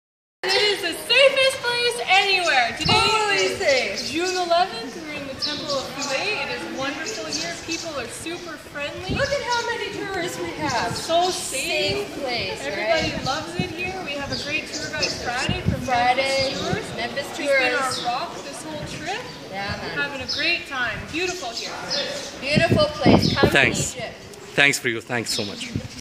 Many tourists at Karnak Temple 11th June 2015!!
it`s not a joke, it`s a real! See how many tourists we have at Karnak Temples in Luxor on 11th June 2015. This`s another real video ensure that life goes as normal in Luxor.
Keep your travel plan and do not worry, it`s very safe to enjoy the ancient history now!